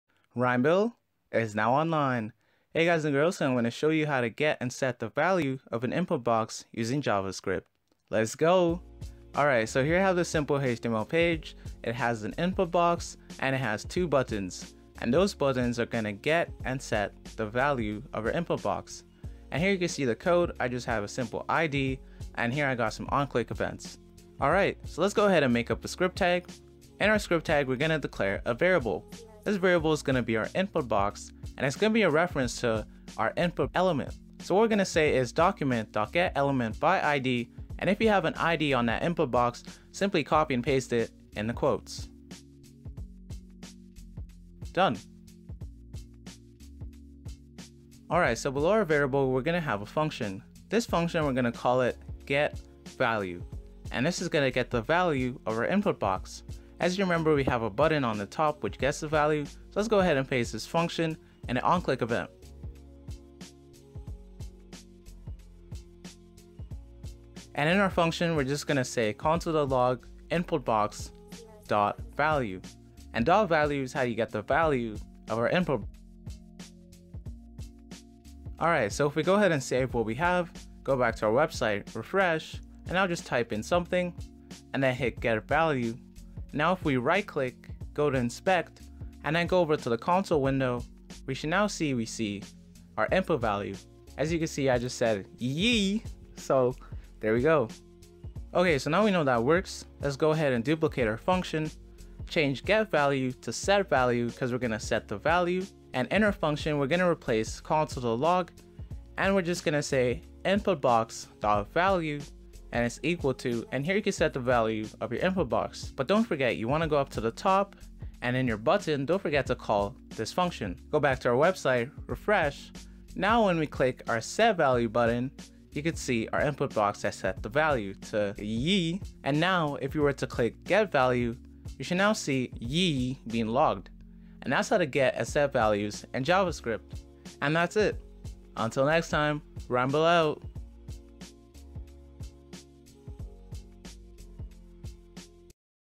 JavaScript: How to SET and GET values in an INPUT BOX
JavaScript: How to SET and GET values in an INPUT BOX. Hey guys and gurls. I'll be showing you how to set and get values in an input box. It's super simple and with a lil RhymBil pizzazz it would be as easy as breathing. Well hope you enjoy it and keep on coding.

Until Next Tine
RhymBil Out